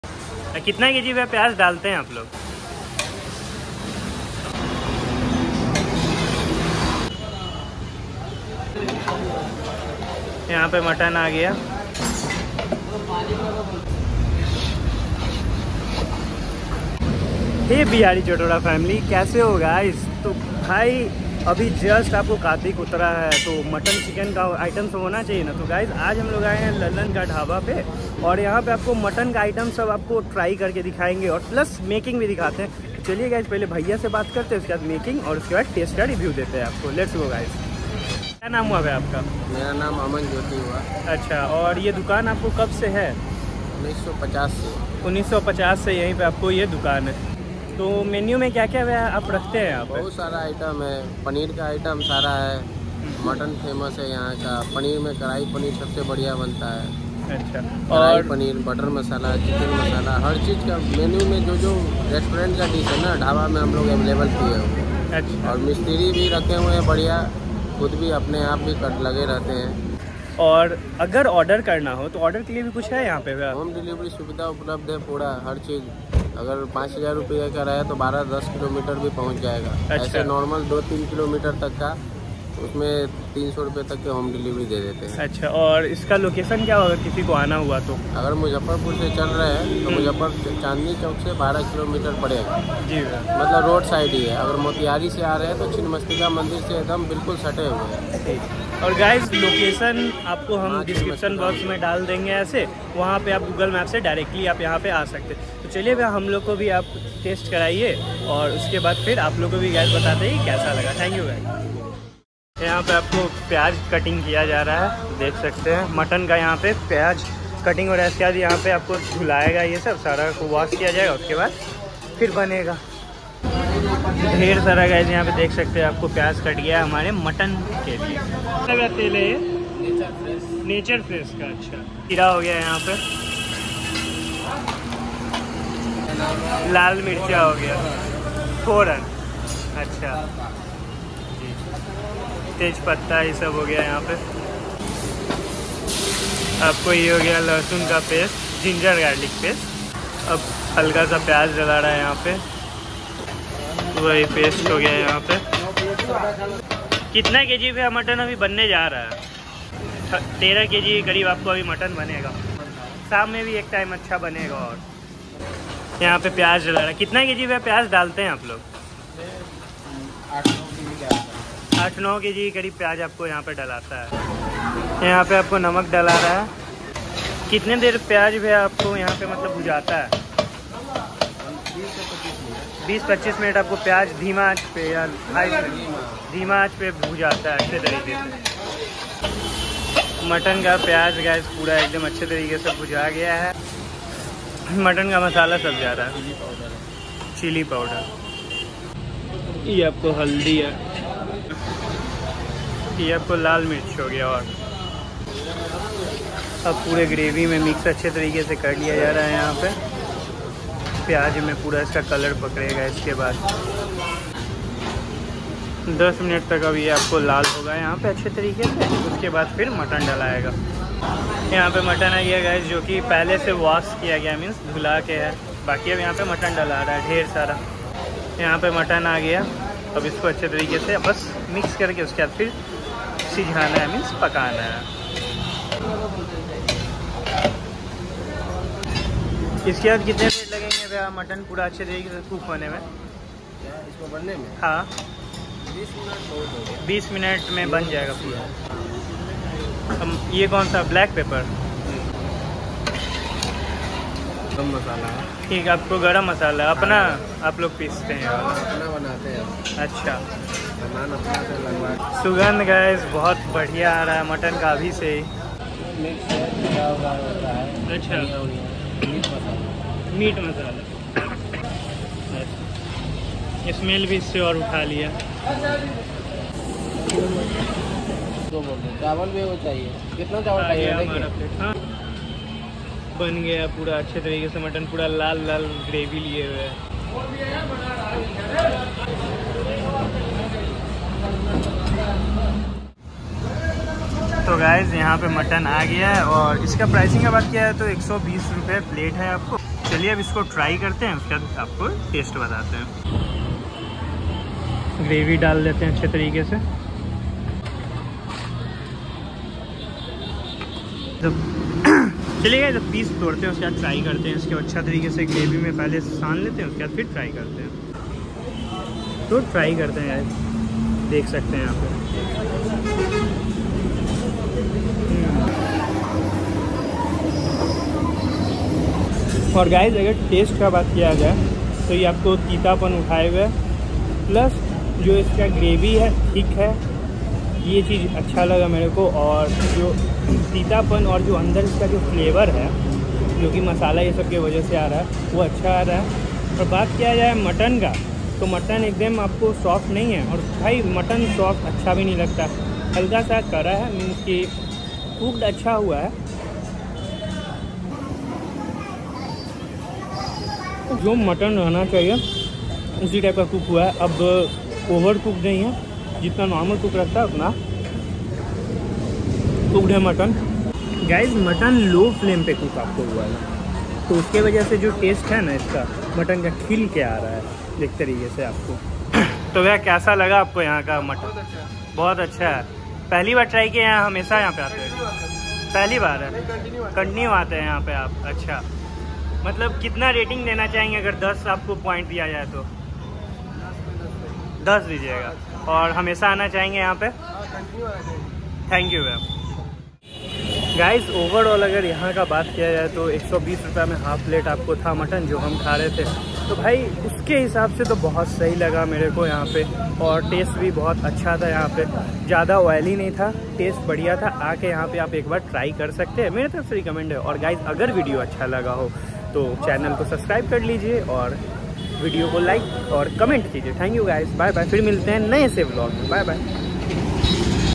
Highway वाले Dabha का Mutton Curry 😍 इतना टेस्टी की उँगली चाटते रह जायोगे 🤤
1 Time try to banta hai yaha ka guys

120 half plate me 2 piece mutton
240 full plate me 4 piece mutton
Location: LALAN DHABA beside maa chhinmastika devi mandir kanti muzaffarpur